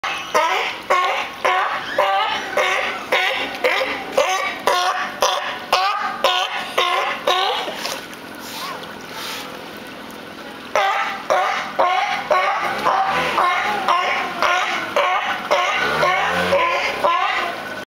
Utica Zoo 09072012 002
This video was taken by Kristine Bellino of WIBX on 07 September 2012 at the Utica Zoo in Utica, New York.  The video features a male sea lion, Porter, and a female, Munchkin, who recently gave birth to a baby named Zoey.  Zoey is not seen in the video.